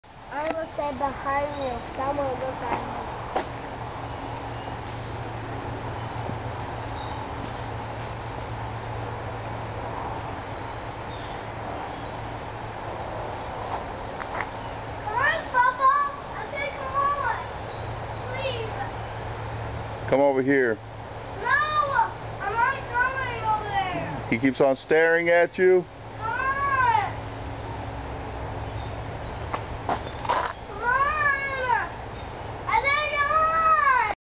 Lion scares child just by looking
When you look into eyes and they kinda tell you something isn't right.  Maybe the eyes tell  you their hungry.  Maybe they tell you, you are invited for dinner, and you know you don't want to be there.
That is the way my son felt when he looked in the lion's  eyes at the Ellen Trout Zoo, Lufkin, Texas.  My son noticed it and said he would stand behind me and stood where we first notice the lion about 20feet away.  As I approached, he stay and yelled for me to come  back.  The lion never quit staring at him.  We spoke of the incident at length and went back to see the lion, who had  moved.